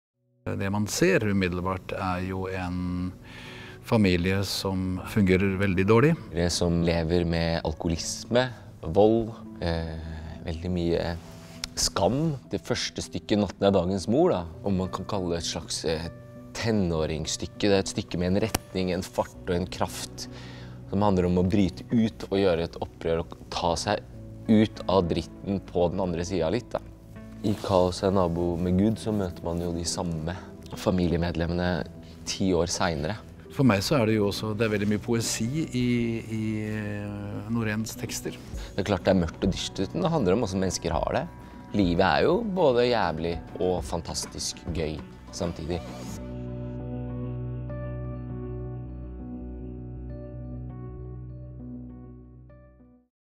Kaos er nabo til Gud
Kaos er nabo til Gud er andre kapittel i Lars Noréns utleverende og selvbiografiske trilogi om en familie i Sverige på 1960-tallet. Ti år har gått siden sist vi møtte familien i trilogiens første kapittel, Natten er dagens mor, som ble en stor publikums- og kritikersuksess da den ble satt opp på Nationaltheatret våren 2015. Også denne gangen er det den prisbelønnede regissøren Kjersti Horn – og de samme skuespillerne – som skal ta fatt på Noréns brutale univers.